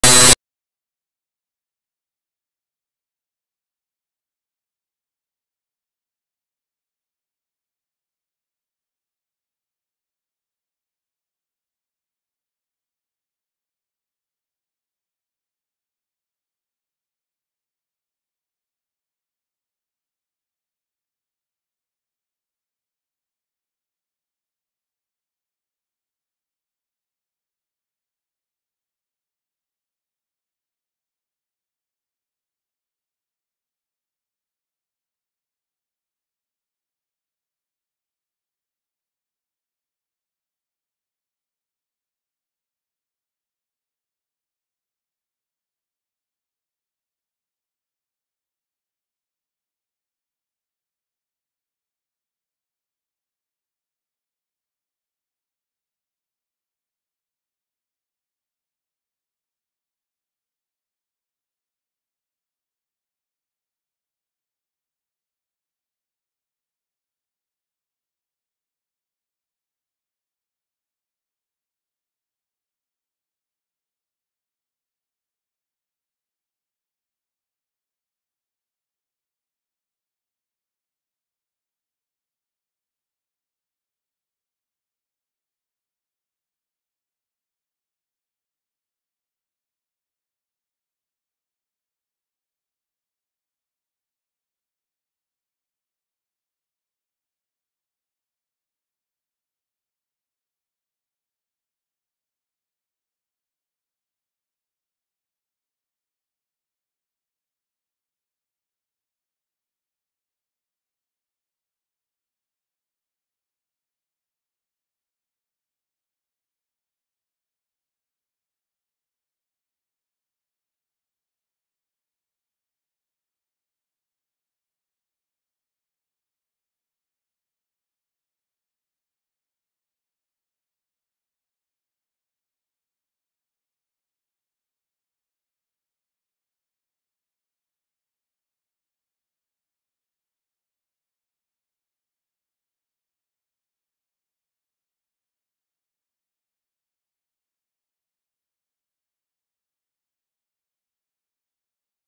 CHRISTMAS MARKET 2006 BRATISLAVA SLOVAKIA part 3
The traditional Christmas market in the Old town of Bratislava, the capital of the Slovak republic. A fine place to meet your friends, have a lot of delicious food and drinks.